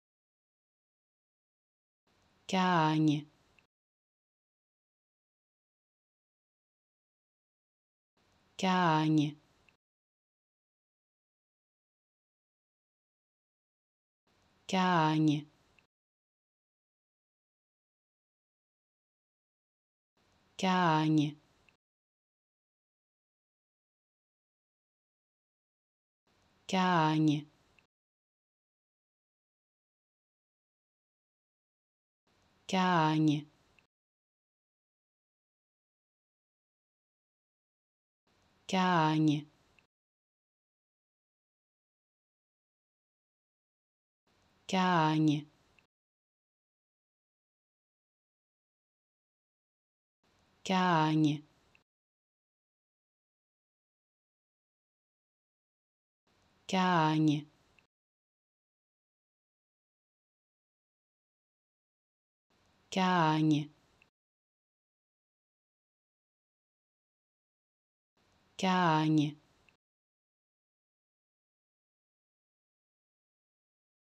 How to Pronounce ''Cahagnes'' (Cahagnes) Correctly in French
Learn how to say and properly pronounce ''Cahagnes'' in French with this free pronunciation tutorial.